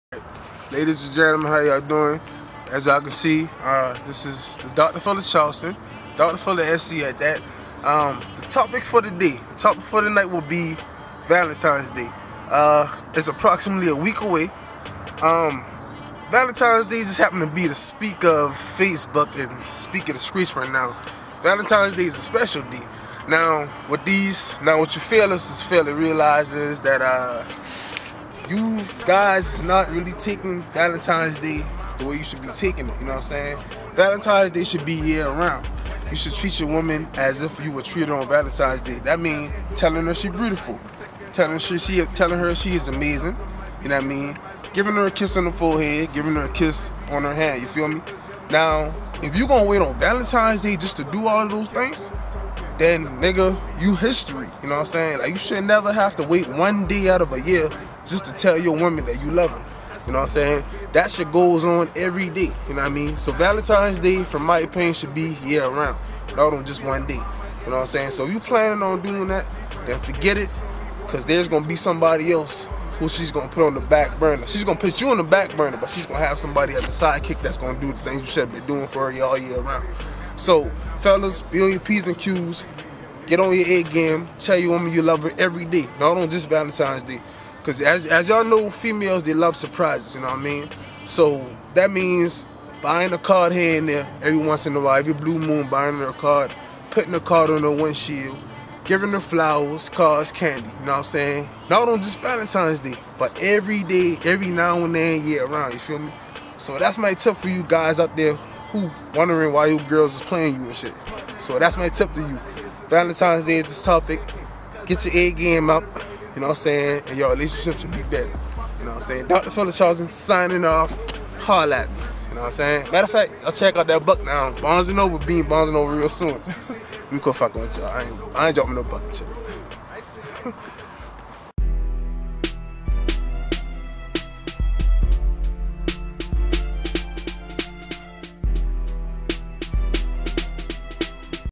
Dr Phil of Charleston "Valentines Day"
Check out the Dr Phil of Charleston as he breaks down Valentines Day for the fellas and the ladies!

follow him @teamtooheavy
follow me @jigalowceo
follow us @nobodygrindsdvd